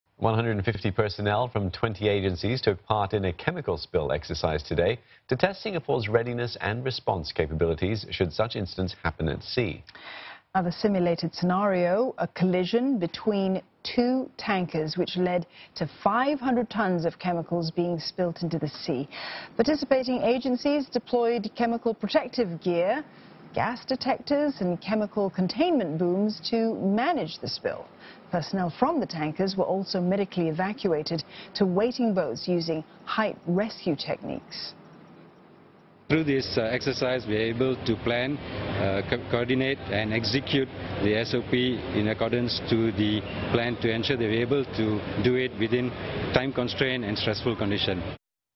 Multi-agency drill tests readiness to manage pollution incidents
One hundred and fifty personnel from 20 agencies took part in a chemical spill exercise on Oct 24, to test Singapore's readiness and response capabilities should such incidents happen at sea.